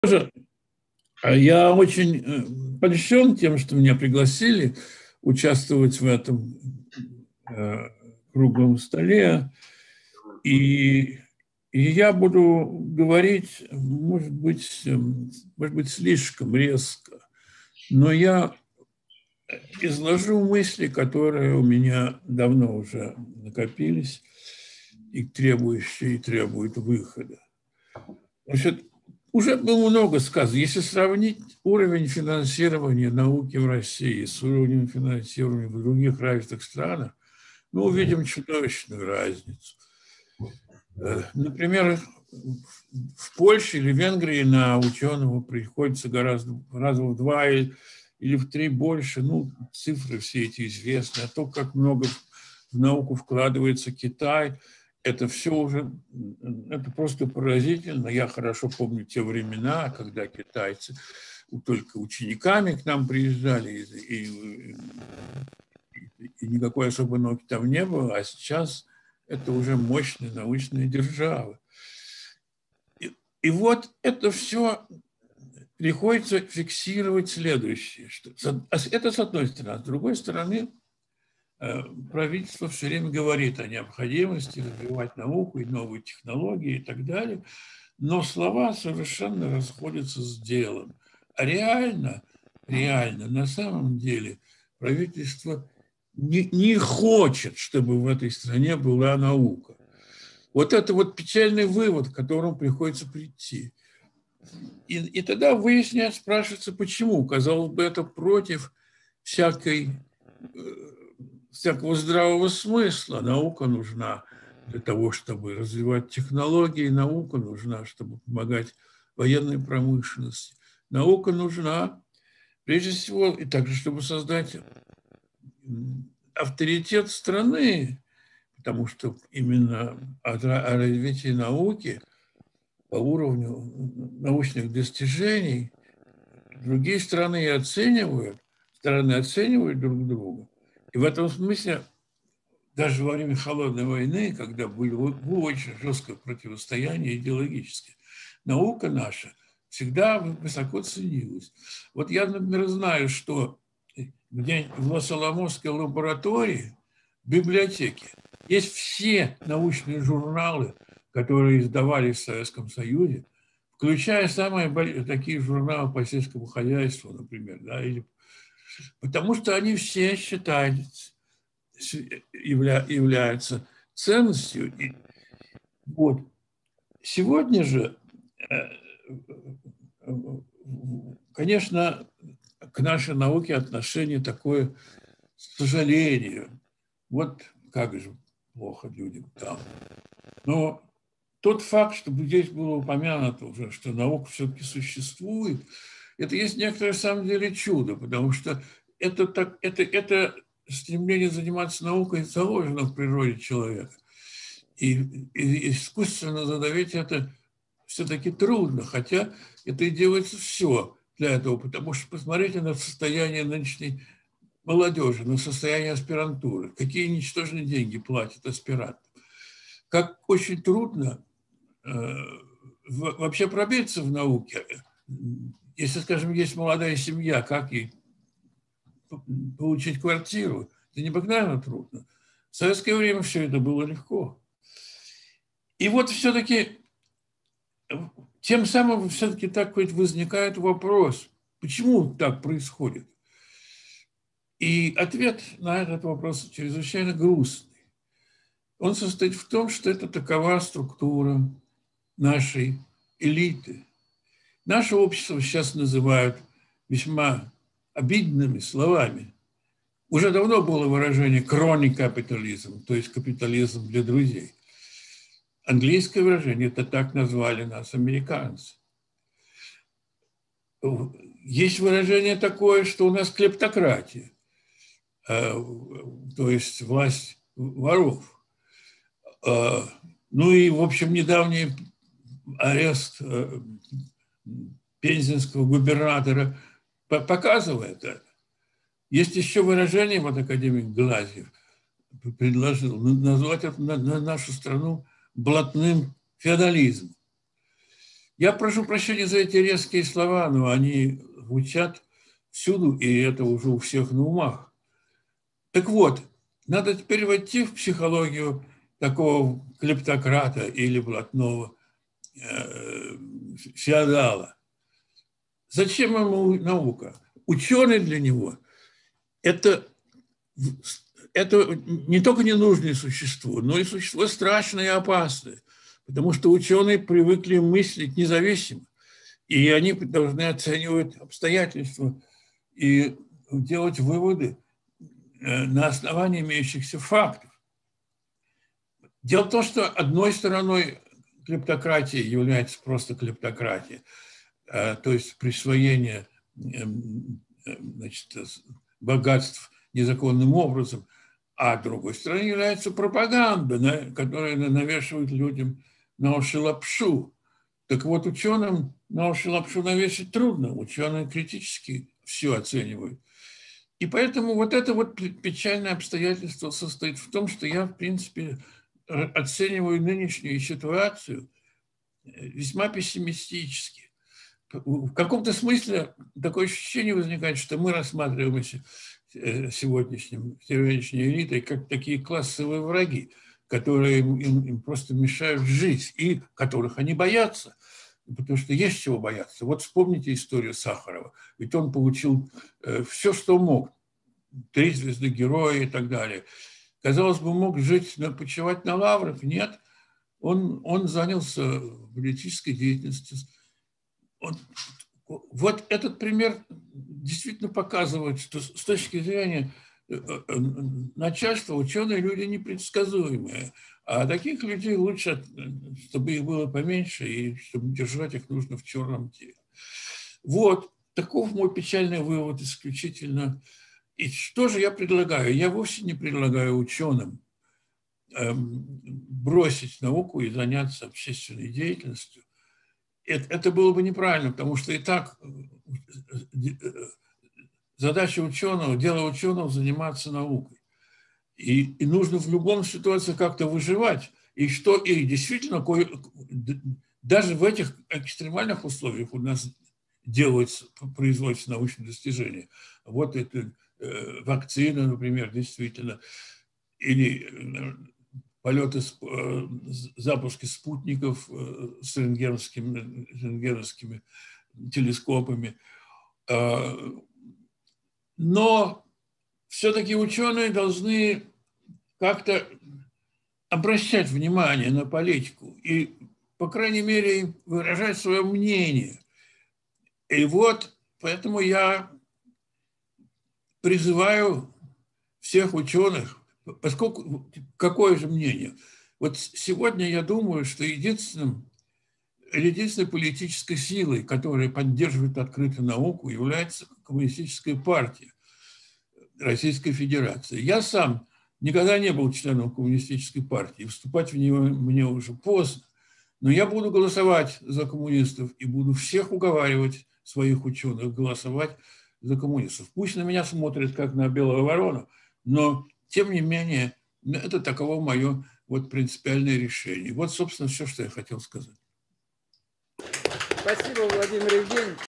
Владимир Захаров на Круглом столе "О кризисном состоянии российской науки и путях выхода из него"
Выступление академика РАН, лауреата Государственных премий СССР и РФ  Владимира Евгеньевича Захарова на Круглом столе " О кризисном состоянии российской науки и путях выхода из него", организованном РУСО, движением "ЗВОН" и профсоюзами.